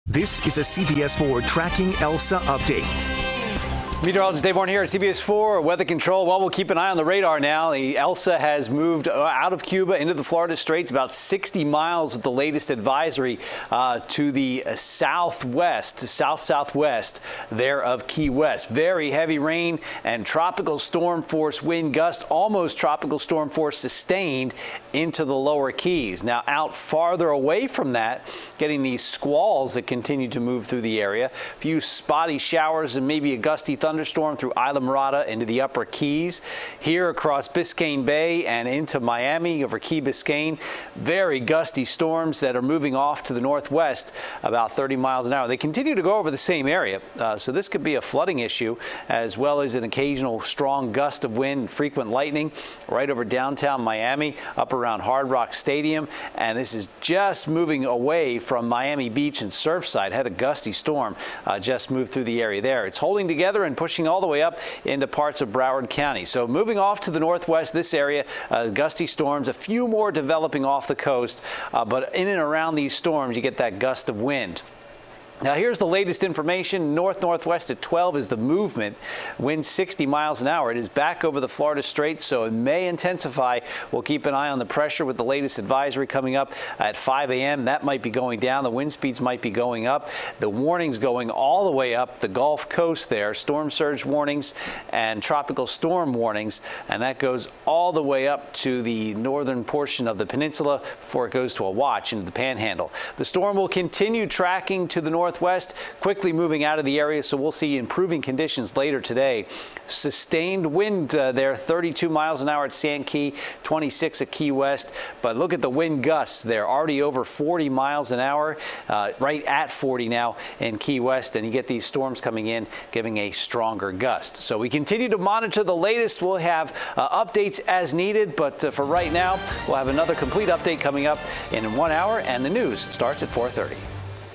Tracking Elsa: Storm Moving At 60MPH
Tropical Storm Elsa is creating spotty showers and gusty storms for South Florida